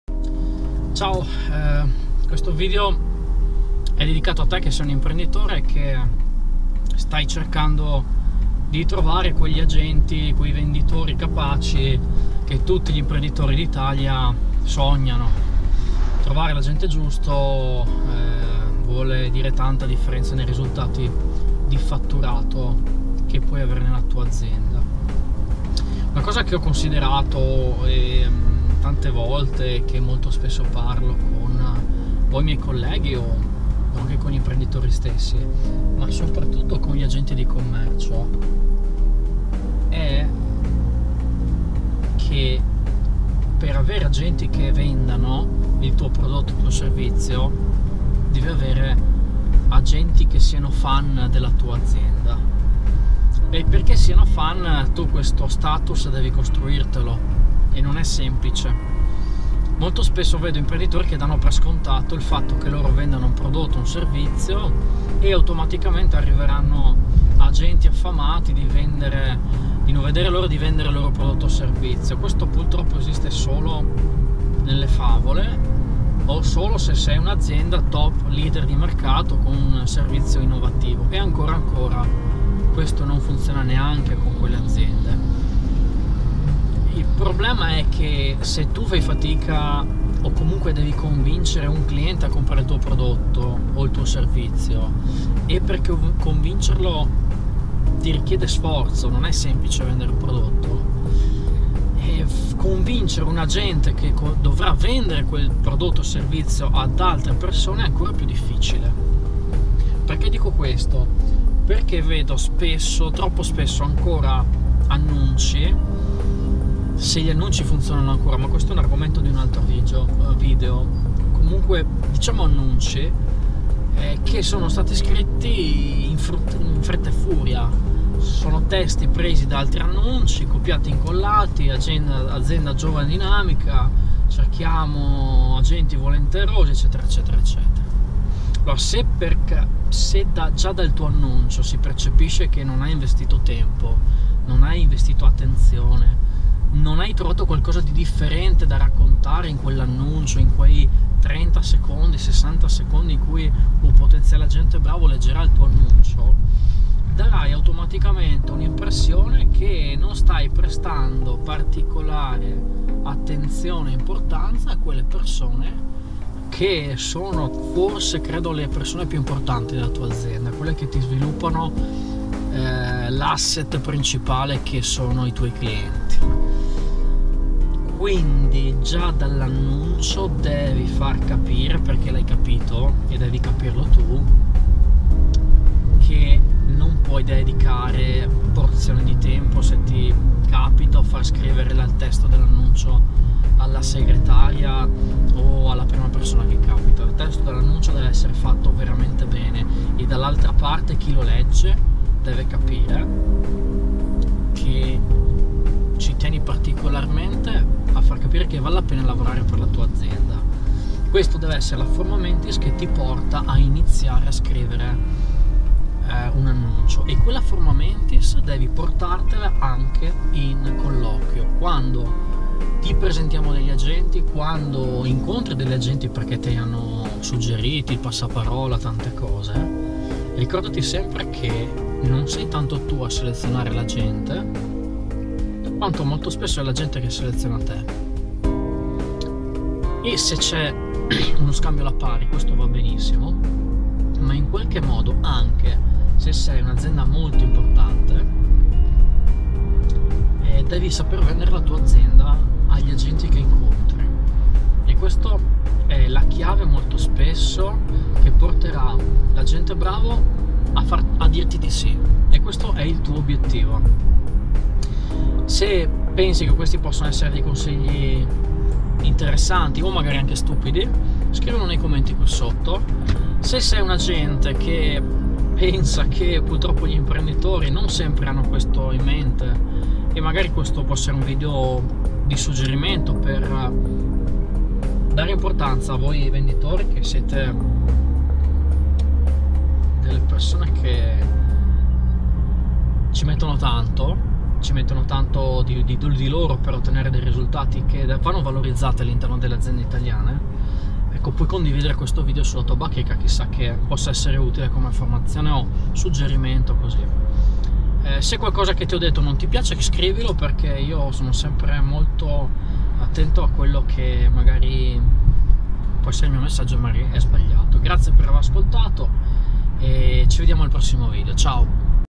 Stai attirando gli agenti e venditori nel modo giusto?
Condivido con te un pensiero su quanto sia importante saper vendere la tua azienda agli agenti che incontri.